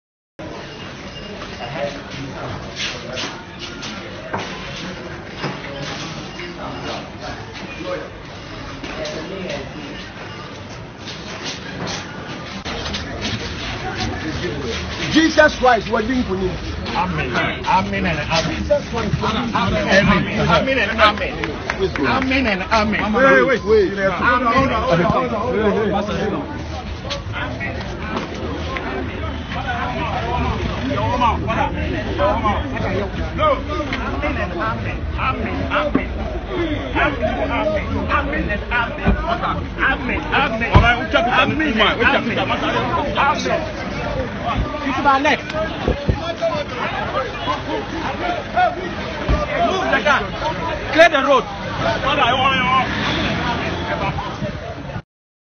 Angel Obinim arrested
Angel Obinim arrested & charged with assault; transferred to Police HQ